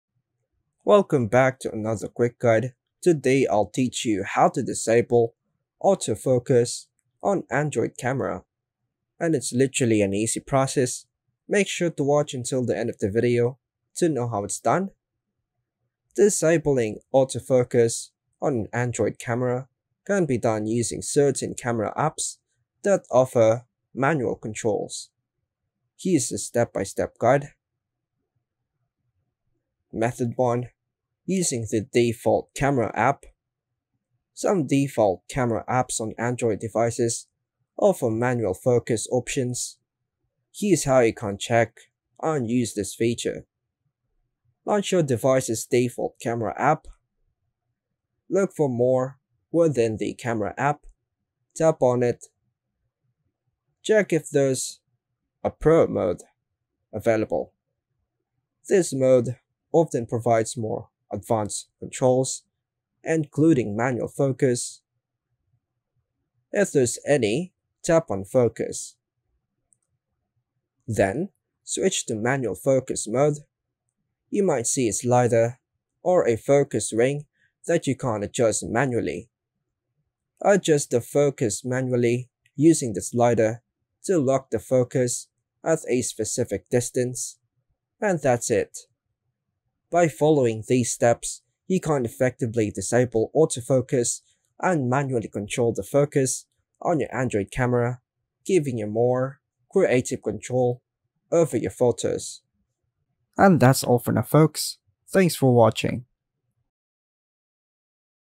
How To Disable Autofocus On Android Camera (Quick & Easy)
This quick guide will show you how to disable autofocus on Android camera. Use this simple change to your camera settings to stop the phone from autofocusing on its own.